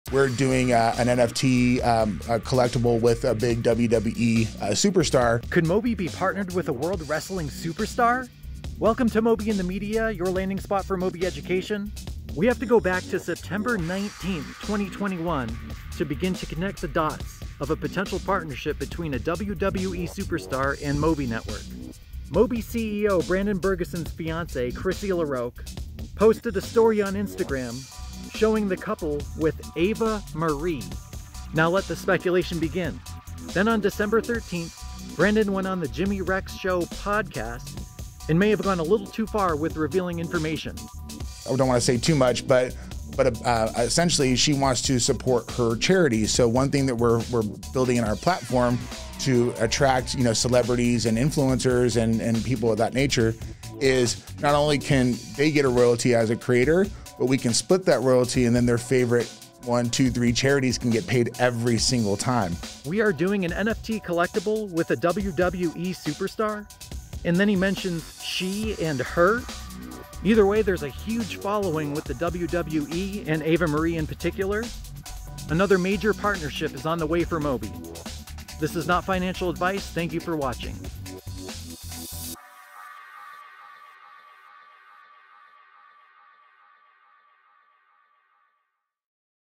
MITM 44: WWE NFTs & Mobie! Is Wrestling Superstar Eva Marie Partnered with Mobie Labs?! (MBX, RAW)
Mobie Labs CEO Brandon Burgason went on the Jimmy Rex Show Podcast and revealed that Mobie (MBX) is working with a World Wresting Superstar on an NFT collectible. Evidence suggests this partnership could involve Natalie Eva Marie, and the information is presented here.